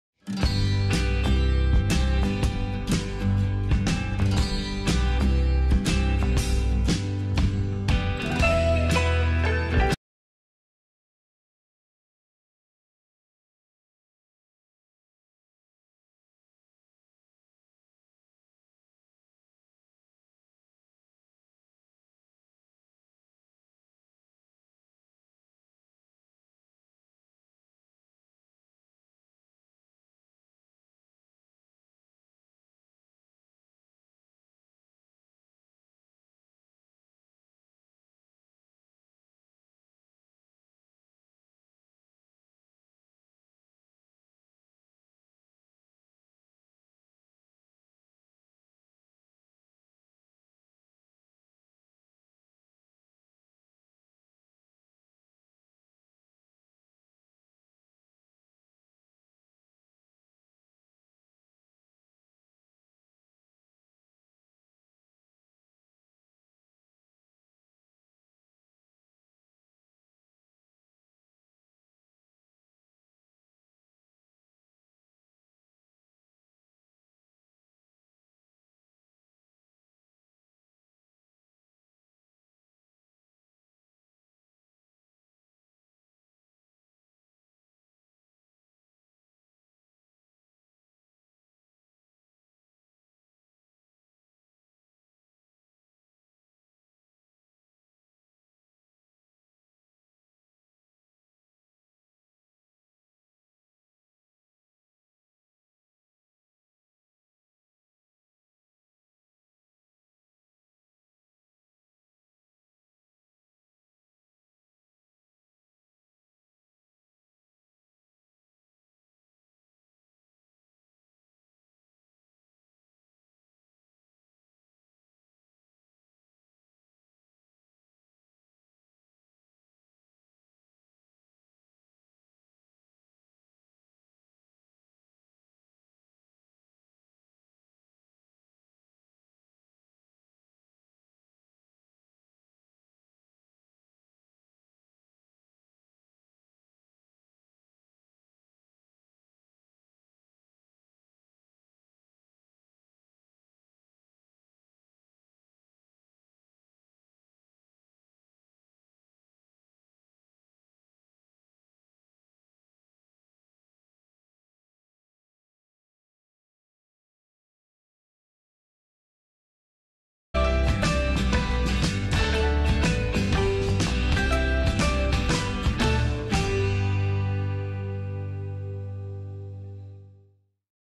Ask the Seed Seller - See How Bayer Packages Research Seed
Purpose: Go to the Urbandale Bayer Site - to see where research plot seed is packaged and the data collection starts!